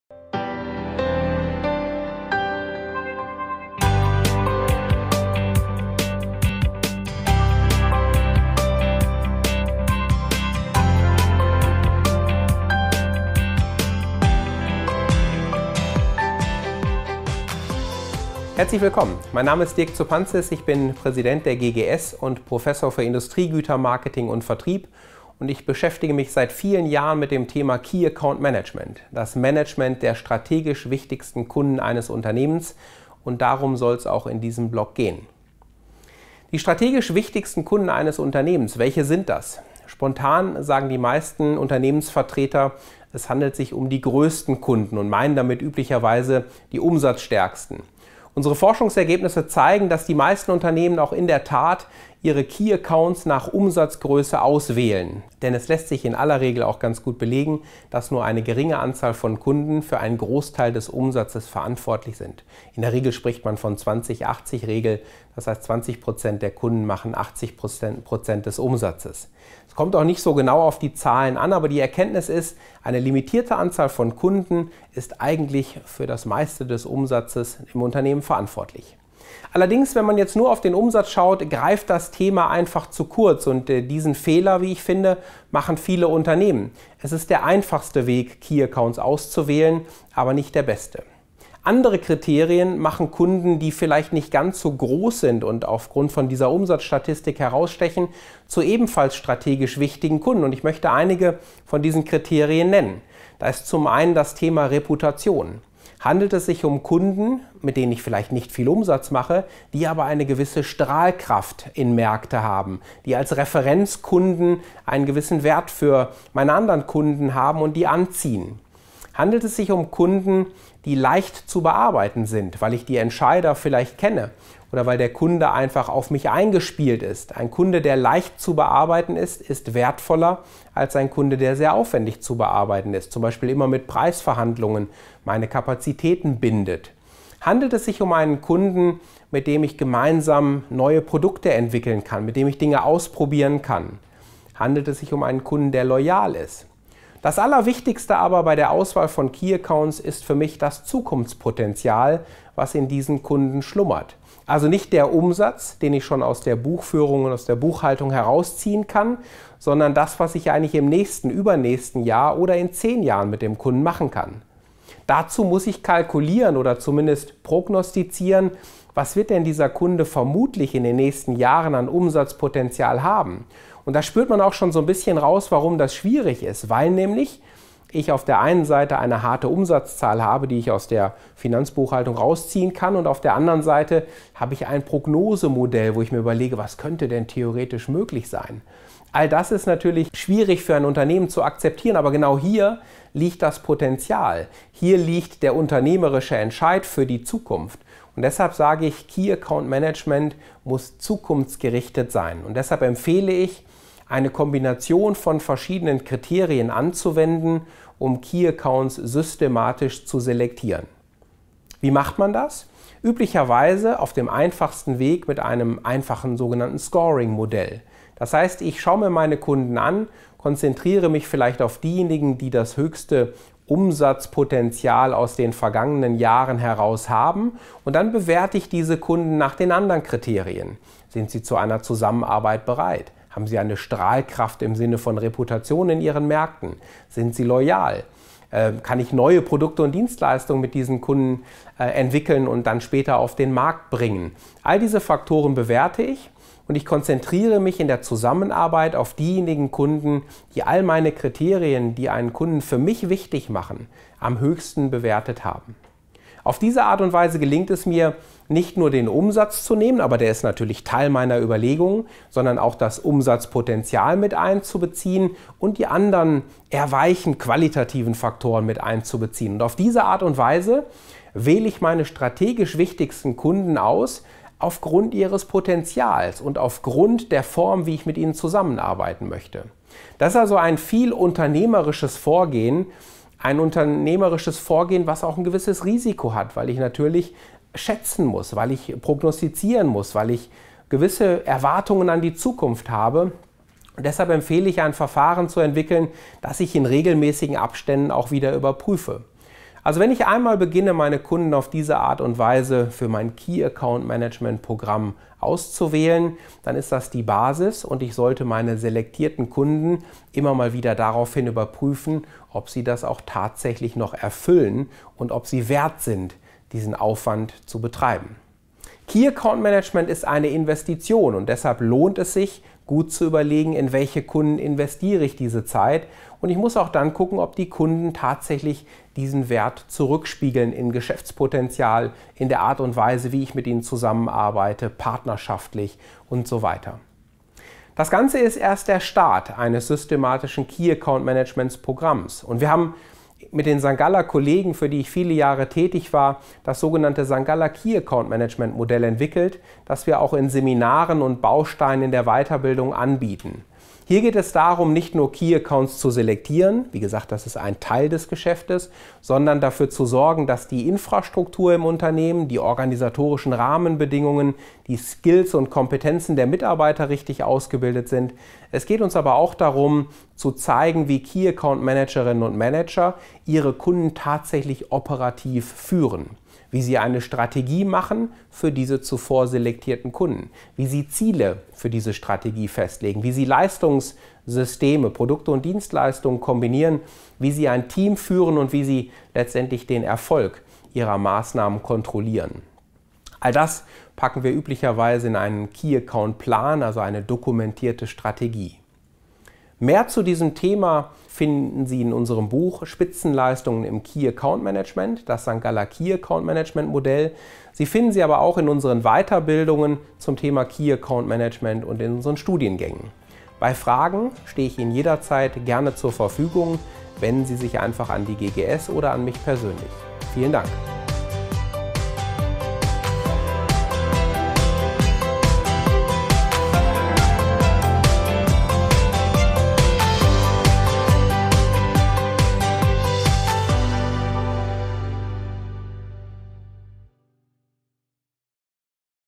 Die richtige Auswahl von Key Accounts
Professionelles Key Account Management beginnt mit der richtigen Auswahl der Key Accounts für das Key Account Management. Welche Kriterien sind entscheidend? Wie können diese bewertet werden? Wie werden die Kriterien in einem Verfahren kombiniert? Antworten gibt es in diesem Videoblog aus der Reihe "GGS Inspirations".

Das erwähnte Buch "Spitzenleistungen im Key Account Management: Das St.Galler KAM Konzept" können Sie hier bestellen